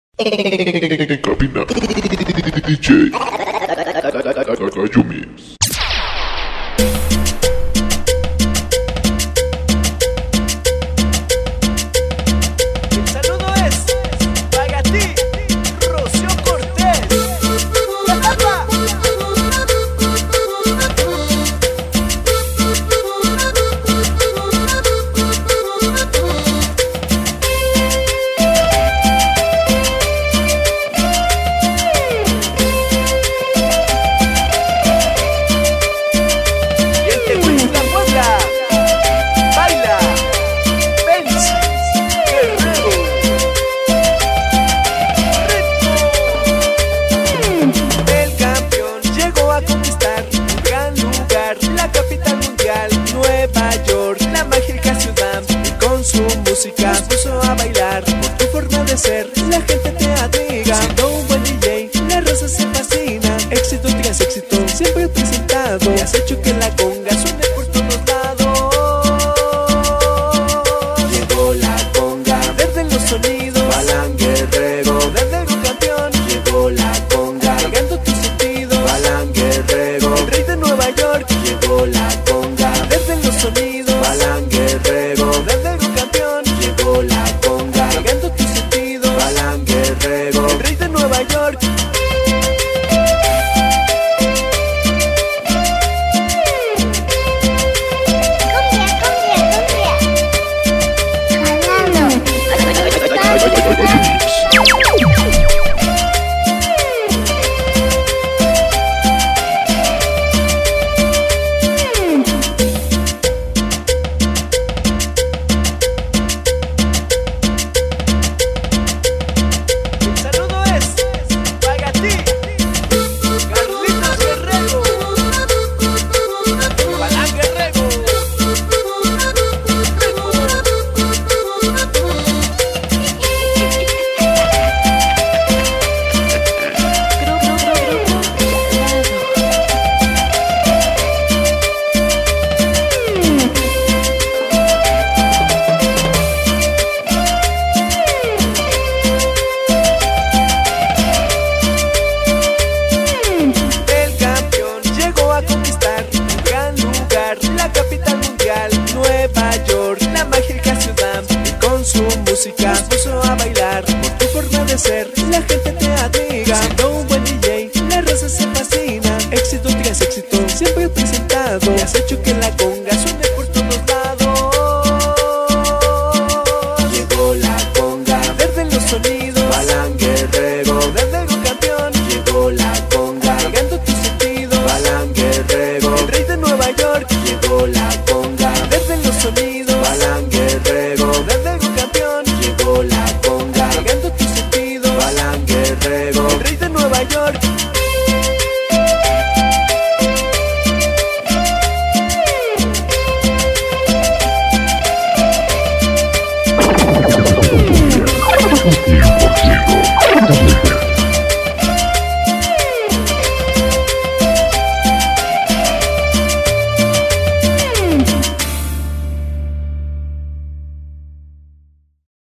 EL CAMPEON JALADO DJ GALLO MIX EN L.A
SIEMPRE UN CAMPEON EN LOS ANGELES CALIFORNIA
DJ GALLO MIX EN CABINA SONIDO GALAKTICO Y MATADOR SALUDOS SONIDO FANTASIA TEHUITZINGO PUEBLA Y PUBLICIDADES LEYKER SIMPLEMENTE!